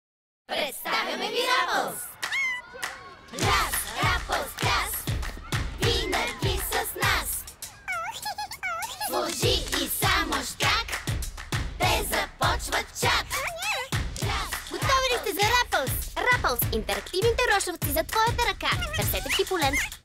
WRAPPLES Интерактивно животинче 28889(28830)
Wrapples са най-сладките и рошави животинчета, които можеш да носиш на ръката си! Сложи ги на китката си и ги носи навсякъде със себе си! Тези очарователни малки приятелчета имат над 50 различни звуци и реакции! Виж как те оживяват върху ръката ти! Техните големи очи светват и отразяват емоцията, която Wrapples изпитва в момента. Забавлявай се безкрай с тези рошави и сладки Wrapples! Събери цялата колекция и направи щуро парти със смеещи се интерактивни животинчета!